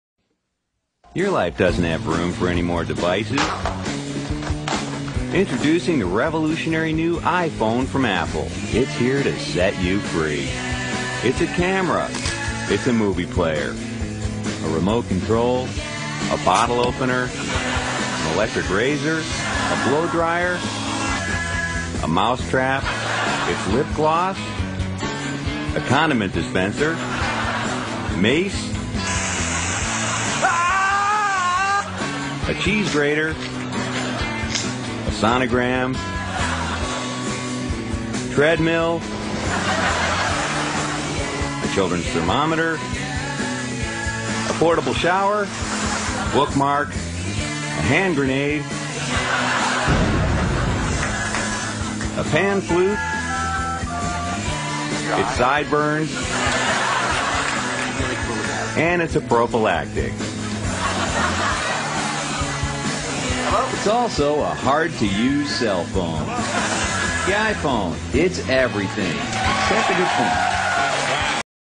iPhone - Camera, Astrolger, MP3 Player
iPhone. It's everything. What iPhone Does - It's A Camera, MP3 Player, Remote, Treadmill, Razor, Mouse Trap, Bottle Opener, Bookmark and also Astrologer